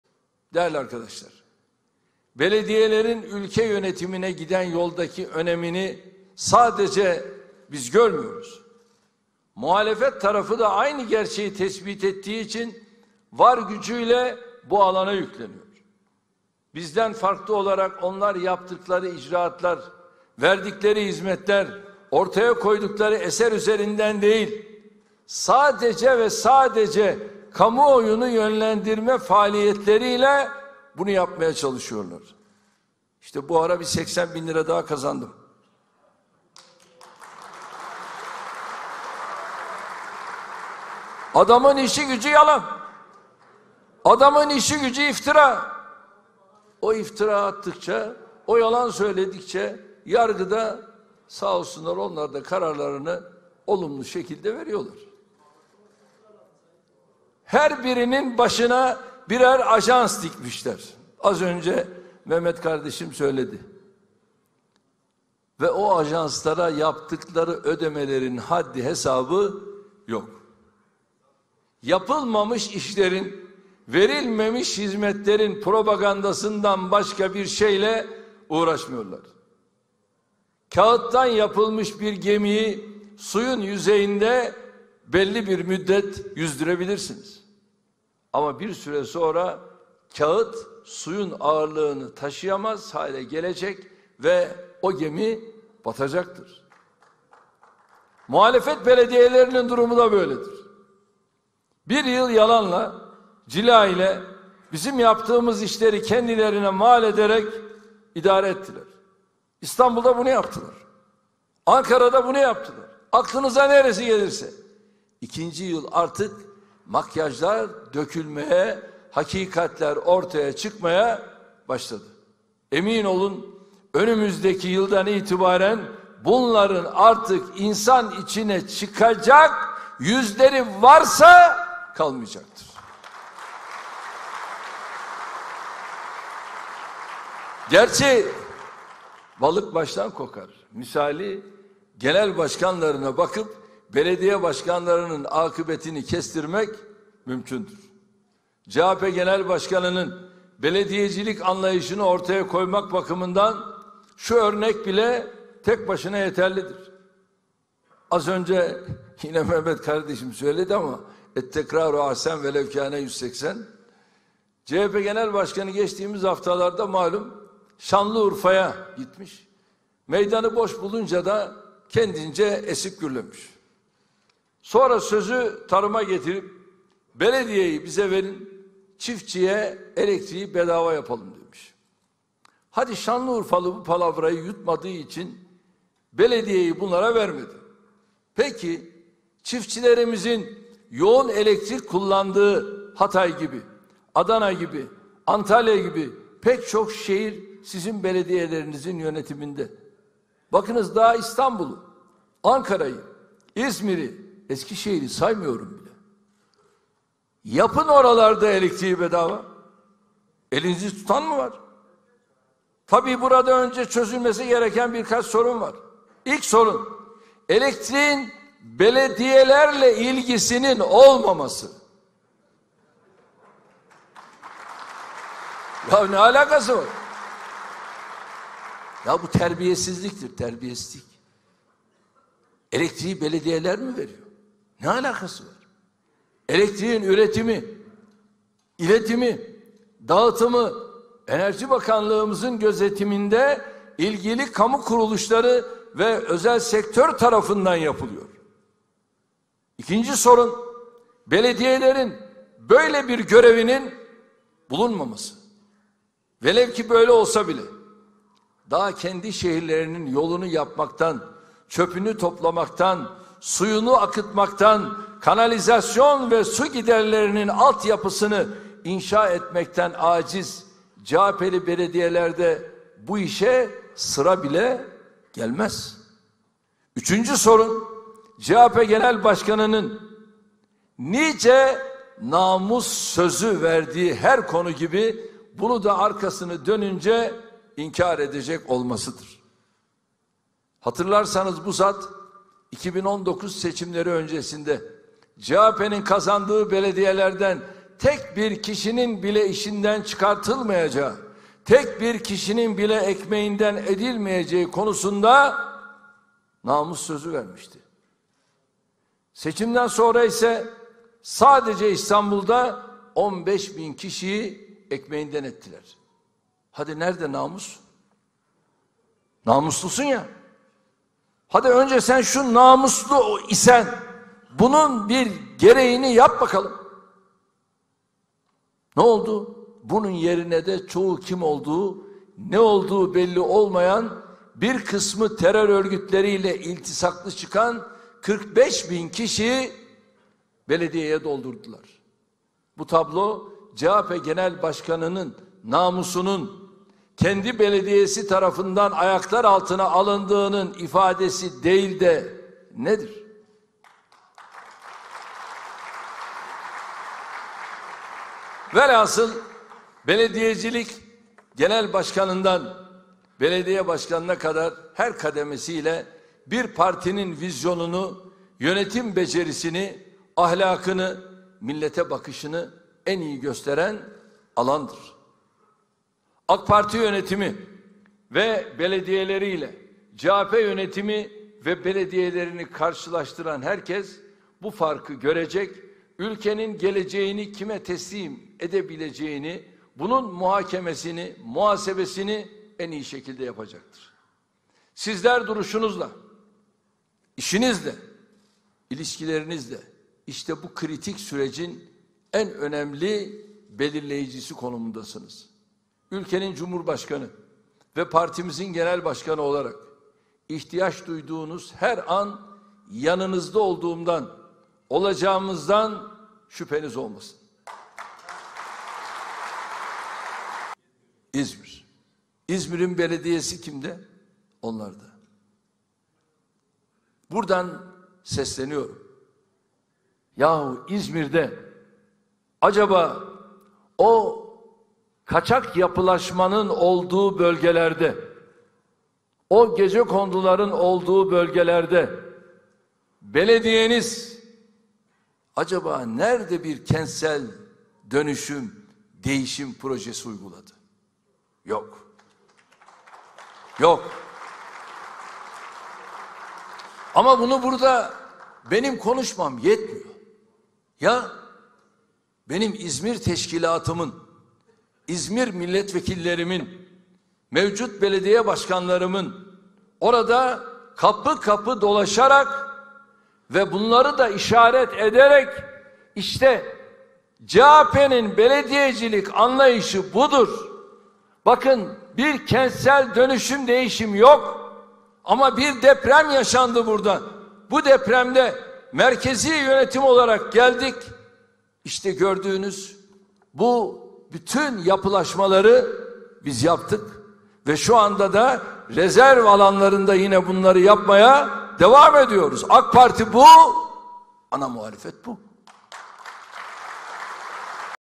Erdoğan, Kılıçdaroğlu'nun NAMUSUYLA OYNADI! Namuslu İsen Gereğini Yap Bakalım
Recep Tayyip Erdoğan, Belediye Başkanları İstişare ve Değerlendirme Toplantısı'nda konuştu.

Yakın tarihimiz bize belediyelerde başarısız olan partinin merkezi yönetimde de başarılı olamadığını göstermiştir. Hangi görevde olursak olalım 81 vilayetin tamamındaki belediyeleri, özellikle kendi belediyelerimizi yakından takip ettik. 30 büyükşehrin 15'i, 519 büyükşehir ilçesinin 304'ü AK Partili kadrolarca yönetilmektedir. Cumhur İttifakı olarak baktığımızda büyük şehirlerde 16 belediyeye çıkıyor. Bu başarıyı arttırarak sürdürmemiz şarttır.

Son seçimlerde CHP'ye geçen belediyelerin şehirlerimize nasıl ağır maliyetler getirdiğini takip ediyoruz. Ne ülkemizi, ne milletimizi, ne de partimizi böyle bir durumla bırakmaya hakkımız yoktur. Hep birlikte daha çok çalışacağız.  Bir duvarın tuğlaları gibi birbirimizi destekleyerek hedeflerimize doğru yürüyeceğiz.